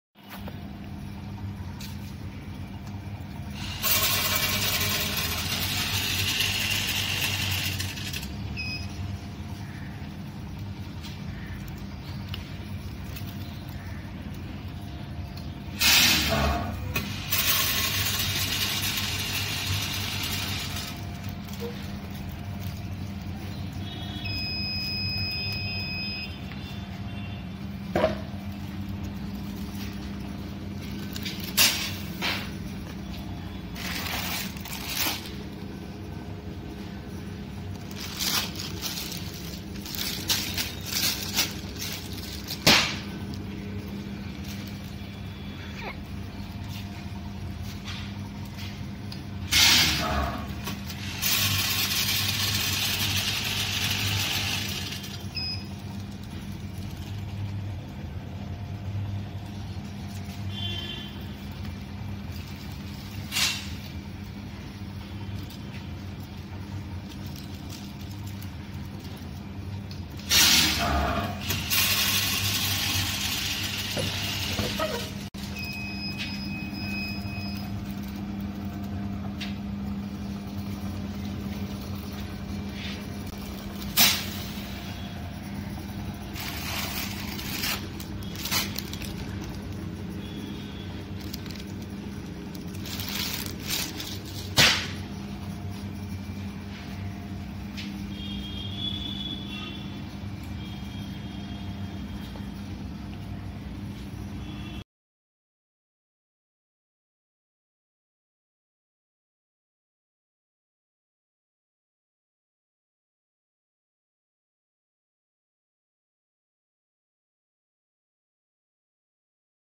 Semi Automatic Weigh Filling Machine | Weigh Filler | Packaging Machine
Semi-Automatic Particle Filling Machine, also known as Granule Filling Machine.. capable of 10g-999g.

Sohan Suthar

Plot No: 02, New Rajeev Colony, Near Sector-58,

Faridabad-121004

Ph No:call or whatsaap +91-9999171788